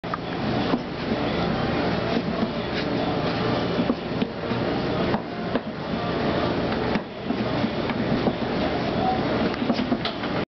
The Dough Hook!
Just a shot of the dough machine at work, this is how dough is churned up at a pizza shop if anyone is interested.